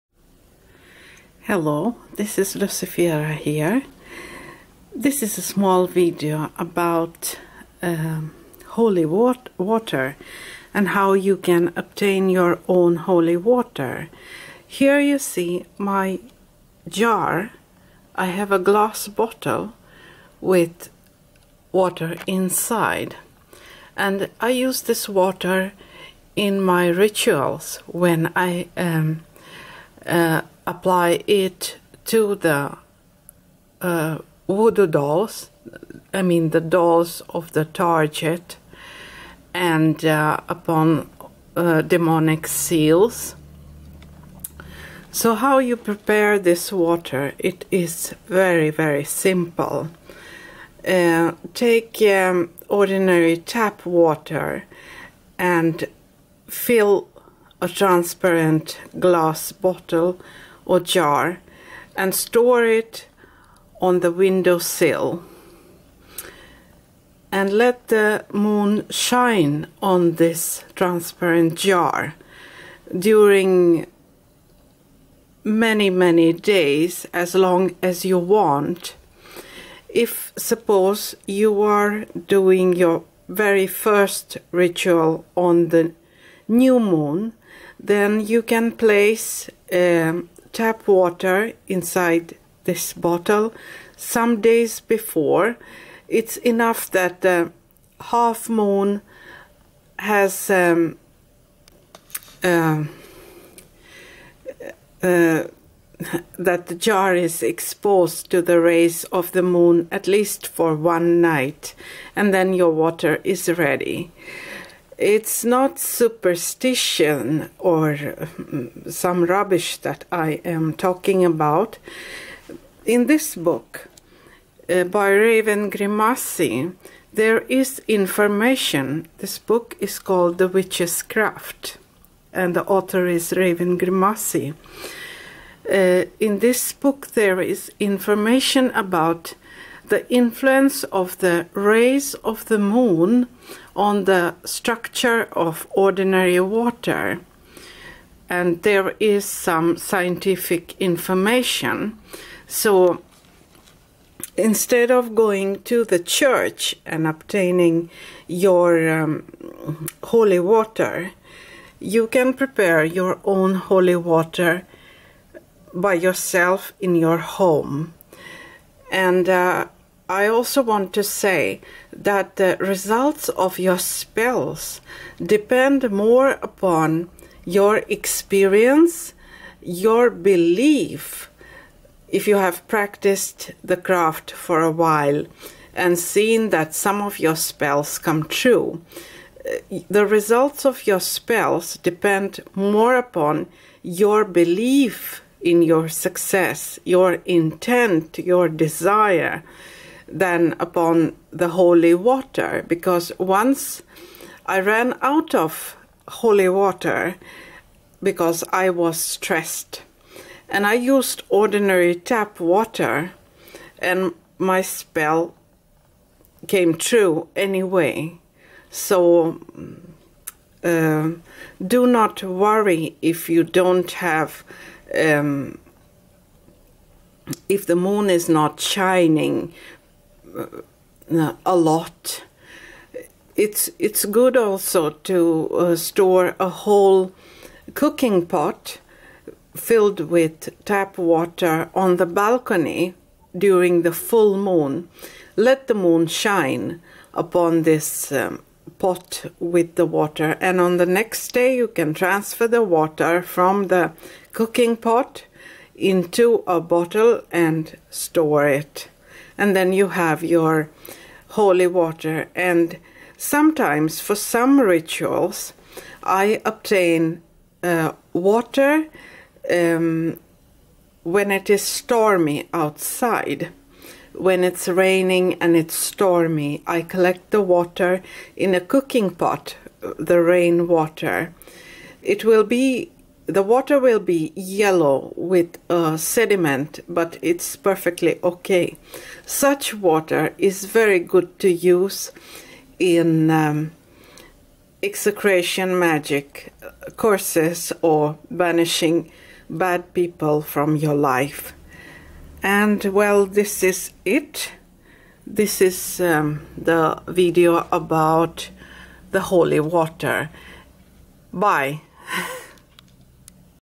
How to prepare Holy Water and Storm Water
How to obtain Holy Water for spells - Beginners guide.